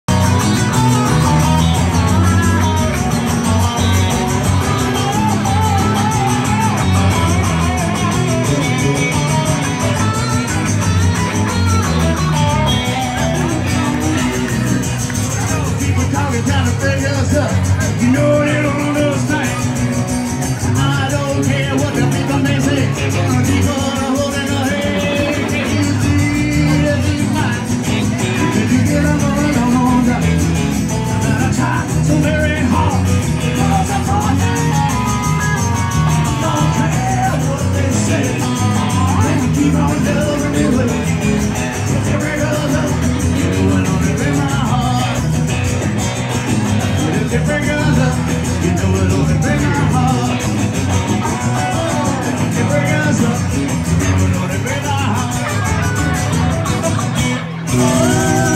Key West Reunion
Pat Guadagno and Al Subarsky bringing Key West to Joe’s Baypoint Inn